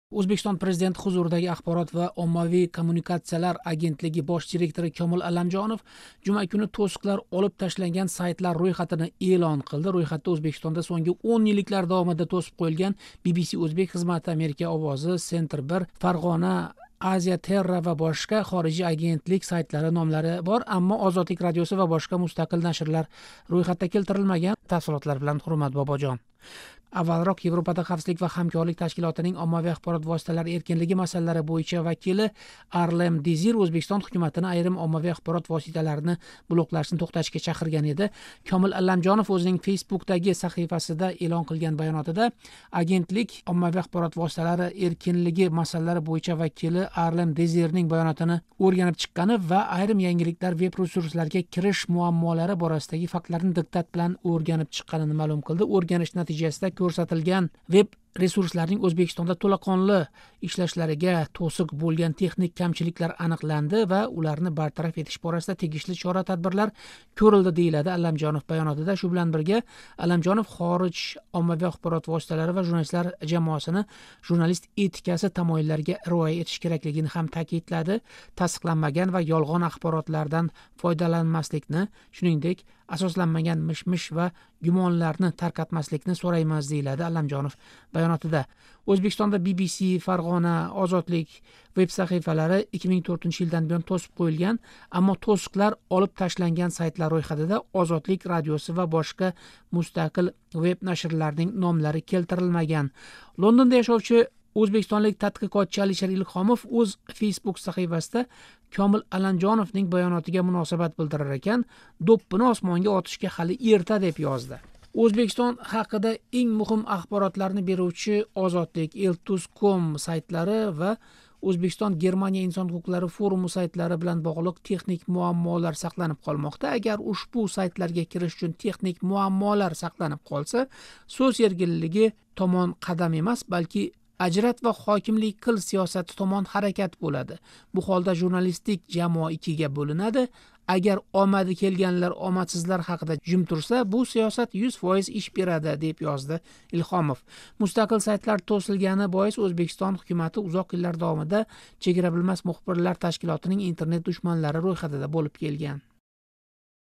Ўзбекистонда айрим хорижий сайтларга тўсиқлар олиб ташланди, аммо улар орасида "Озодлик" йўқ
Ўзбекистон президенти ҳузуридаги Ахборот ва оммавий коммуникациялар агентлиги бош директори Комил Алламжонов жума куни тўсиқлар олиб ташланган сайтлар рўйхатини эълон қилди.

Рўйхатда Ўзбекистонда сўнгги ўн йилликлар давомида тўсиб келинган "Би-Би-Си" Ўзбек хизмати, "Америка овози", "Centre1", "Ferghana Agency", "AsiaTerra" ва бошқа хорижий янгилик сайтлари номлари бор, аммо "Озодлик" радиоси ва бошқа мустақил нашрлар рўйхатда келтирилмаган.

Ўзбекистон президенти ҳузуридаги Ахборот ва оммавий коммуникациялар агентлиги бош директори Комил Алламжонов айрим хорижий янгилик сайтларига қўйилган тўсиқлар олиб ташланганини Facebook саҳифасида эълон қилди.

Бугундан эътиборан “Voice of America”, “Amerika ovozi”, “BBC Uzbek”, “Deutsche Welle”, “Amnesty International”, “Human Rights Watch”, “Reporters sans frontieres”, “Eurasianet.org”, “AsiaTerra”, “Fergana Agency”, “Centre1”, “Uzmetronom” каби веб-ресурслар ва бошқа хорижий веб-сайтлардан тўлиқ фойдаланиш имкони мавжуд бўлади”, — дейилади баёнотда.